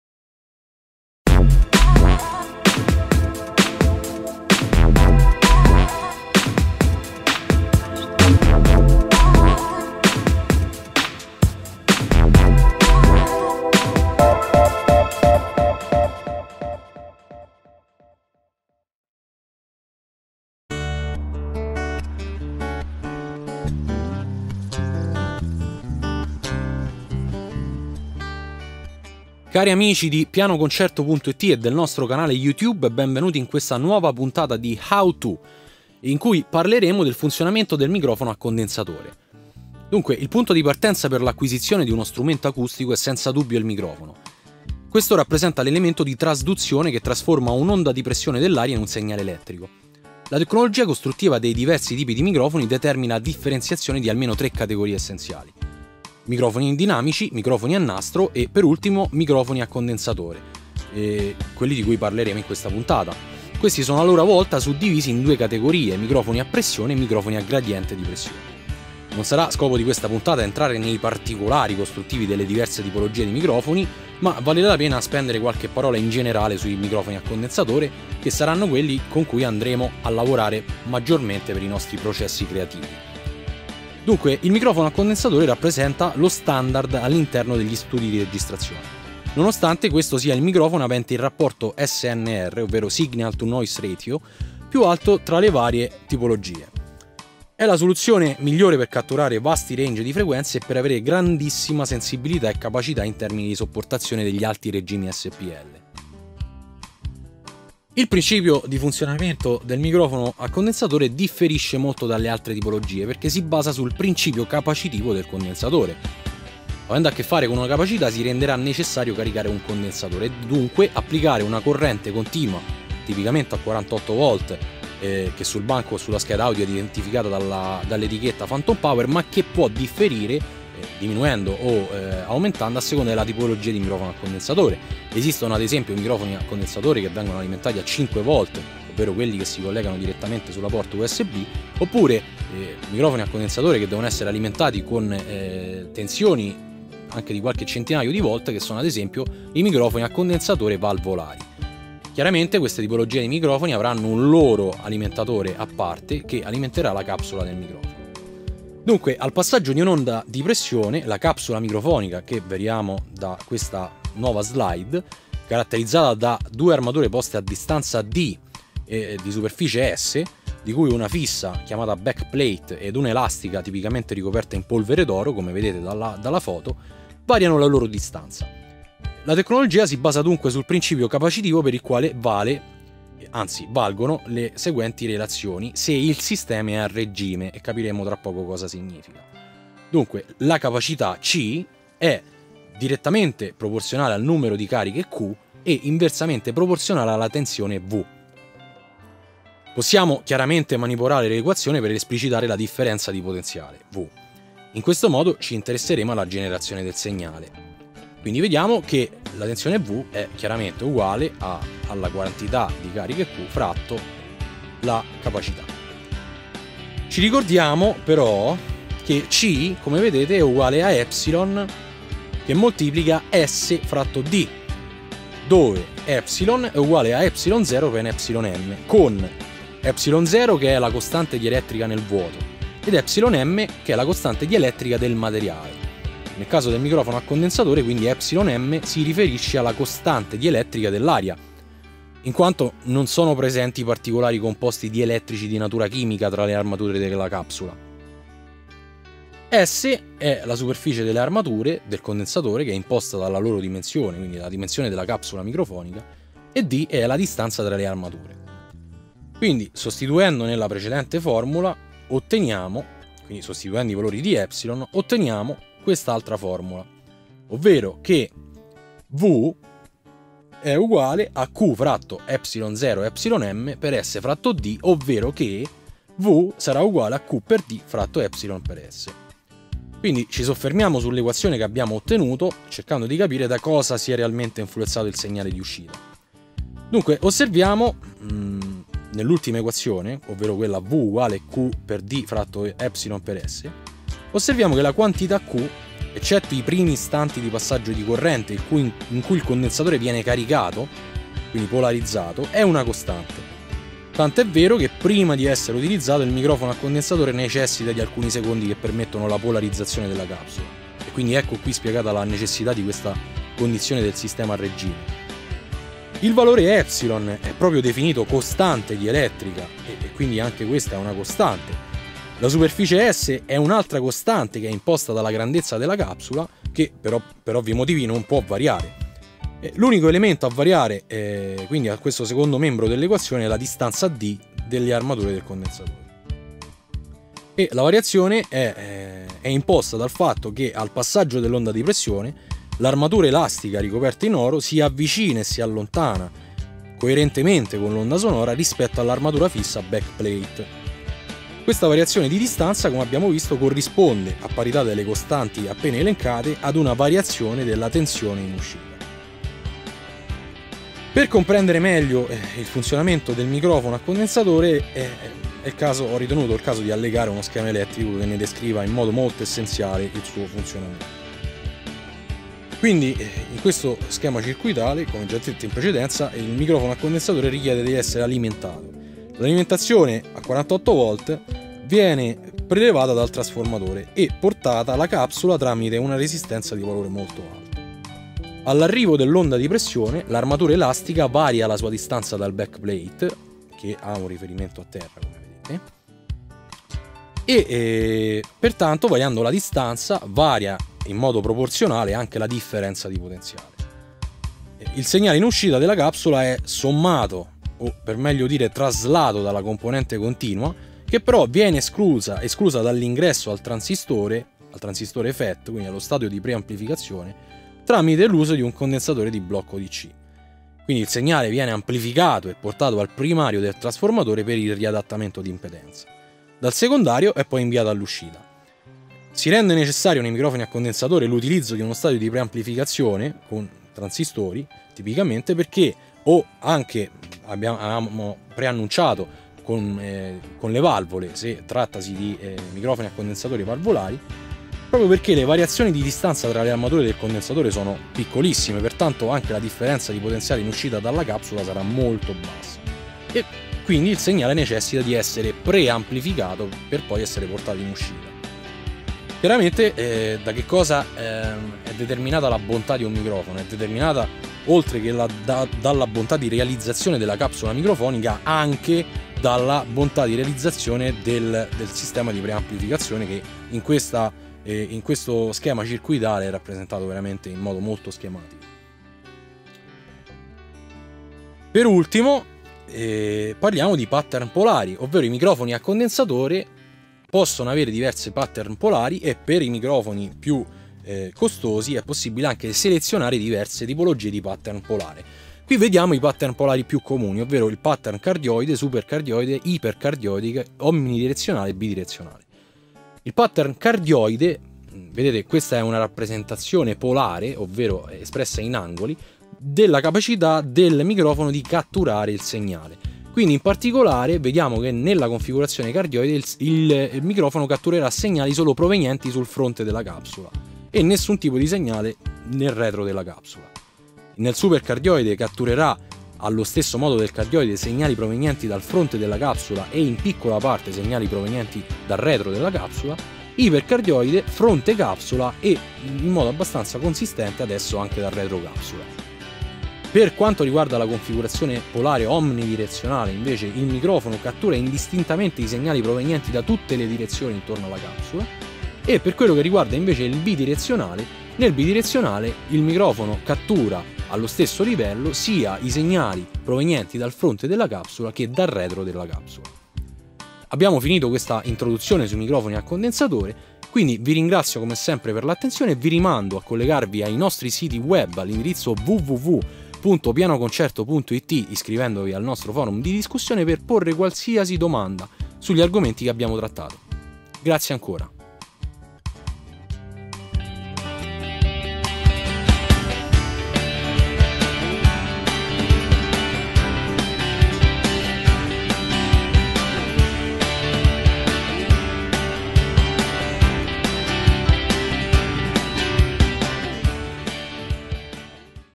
Funzionamento dei microfoni a condensatore - HOW TO by Pianoconcerto.it
Puntata di How to... by Pianoconcerto.

Funzionamento dei microfoni a condensatore

- Tipologie di microfoni
- Microfono a condensatore
- Capsula microfonica
- Proprietà del condensatore
- Elettronica del microfono a condensatore
- Pattern polari dei microfoni

Realizzato a cura di Simone Renzi